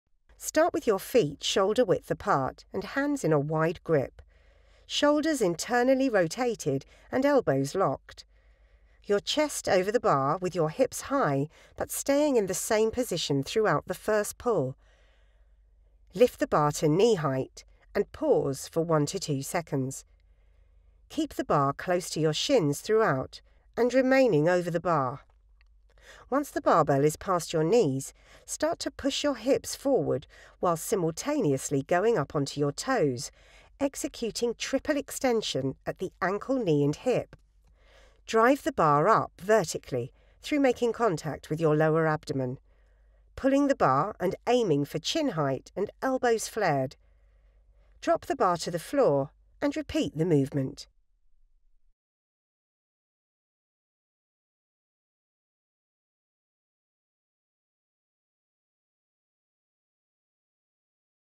Pause Mid Block Snatch Pull | Olympic Lifting | Strength and Conditioning Exercises
Start with your feet shoulder width apart and hands in a wide grip, shoulders internally rotated and elbows locked. Your chest over the bar with your hips high but staying in the same position throughout the first pull, lift the bar to knee height and pause for 1-2 seconds. Keep the bar close to your shins throughout and remaining over the bar. Once the barbell is past your knees start to push your hips forward while simultaneously going up onto your toes, executing triple extension at the ankle, knee and hip. Drive the bar up vertically through making contact with your lower abdomen, pulling the bar and aiming for chin height and elbows flared. Drop the bar to the floor and repeat the movement.